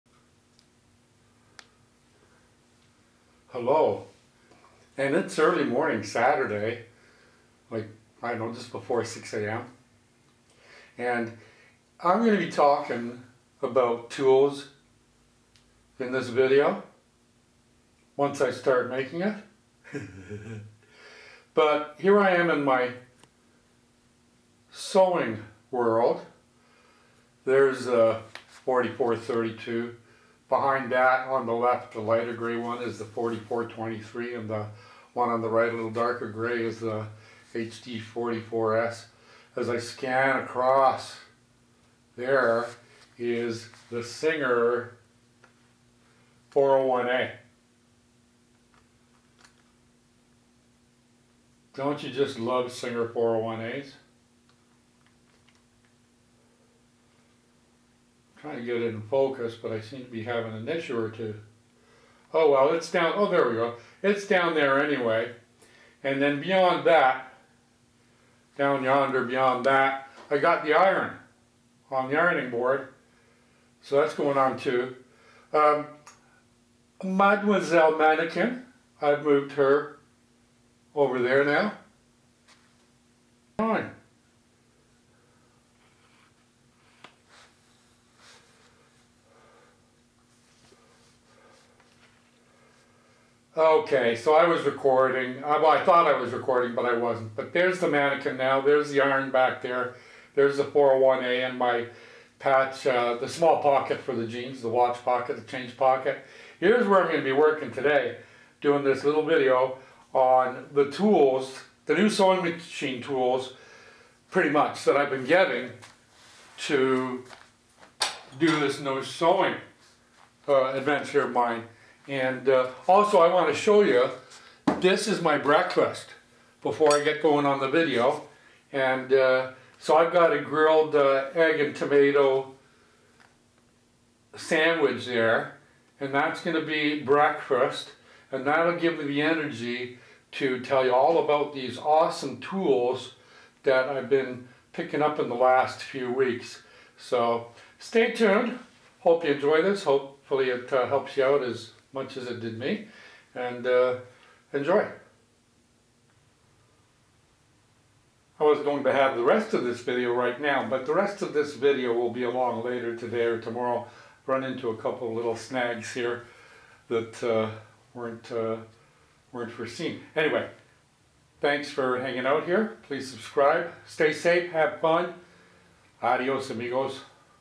SEWING TOOLS
SCISSORS.

00:00 HELLO! AND IT'S EARLY MORNING SATURDAY, LIKE I DON'T KNOW; BEFORE 6AM. I AM GOING TO BE TALKING ABOUT TOOLS IN THIS VIDEO. ONCE I START MAKING IT, LOL! BUT, HERE I AM IN MY SEWING WORLD.
00:50 DON'T YOU JUST LOVE THE SINGER 401A?
01:05 DOWN YONDER, BEYOND THAT I GOT THE IRON! ON THE IRONING BOARD.
01:45 THERE IS THE WATCH POCKET FROM THE JEANS!
02:30 I HOPE THIS HELPS YOU AS MUSH IT HAS HELPED ME.
02:45 STAY SAFE HAVE FUN! ADIOS AMIGOS!